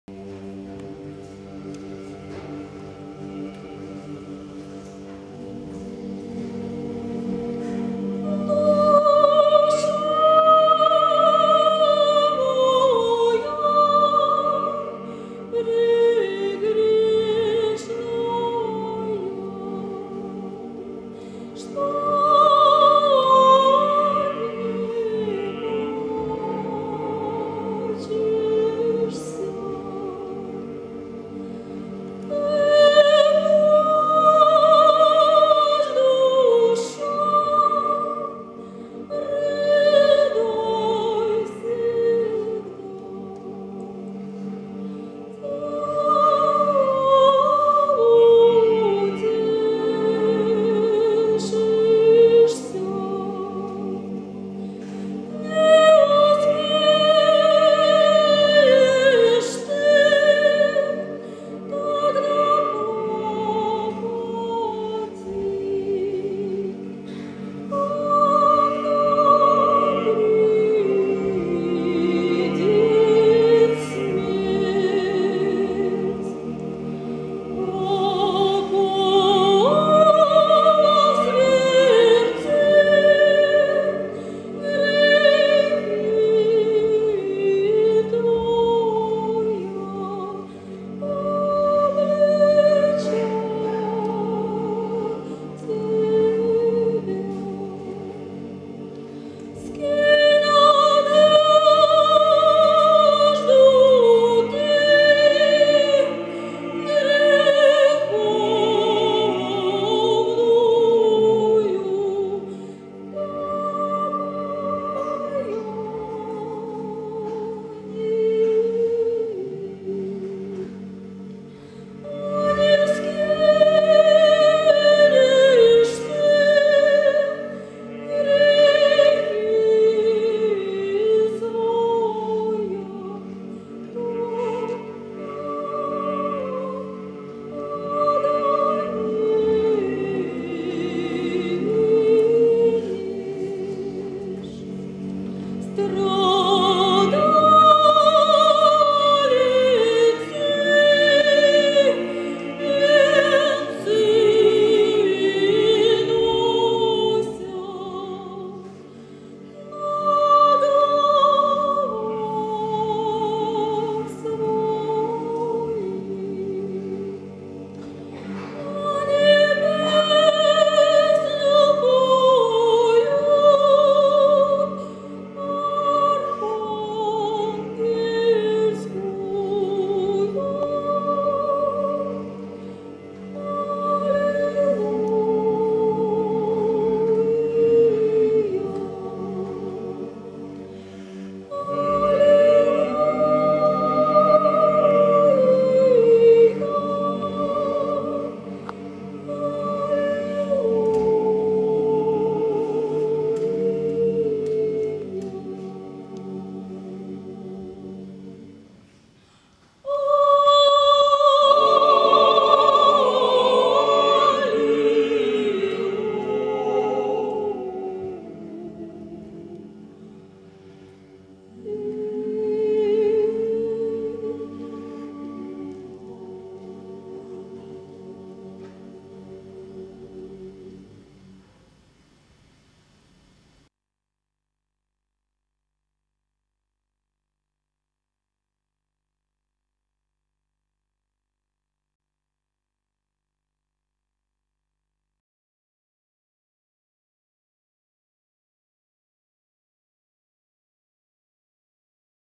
CHOEUR KEDROFF ( ST PETERSBOURG) À CHARLY/MARNE
Extrait d'un concert à l'église de Charly (Aisne) avec insertion d'images prises lors d'un reportage en Russie / Images deJean-Paul Veillard pour ACTU 02 TV